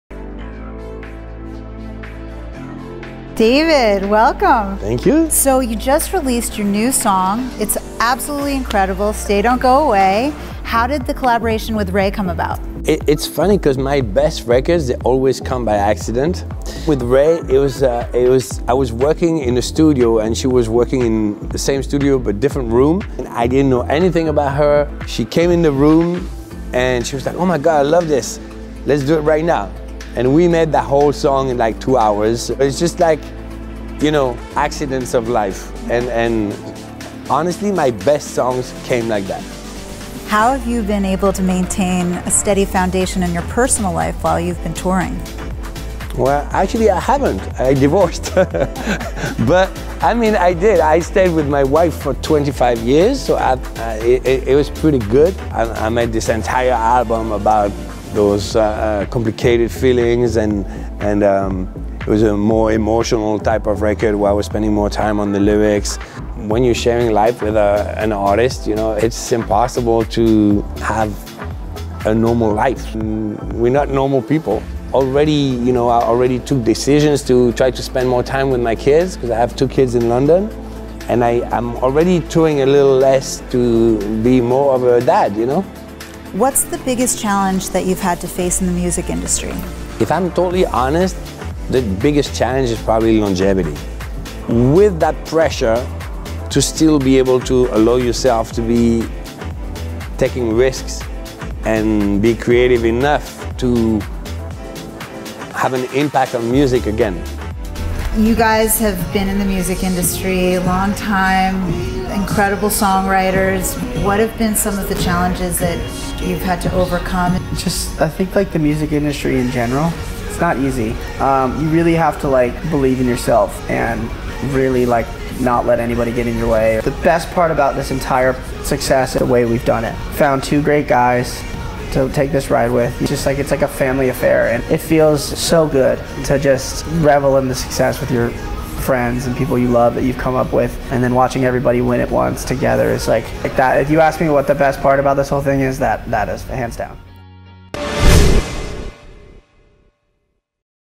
Wendy Starland Presents: David Guetta & lovelytheband
Wendy Starland sits down with David Guetta and lovelytheband to talk music, personal life, and stories from life on tour.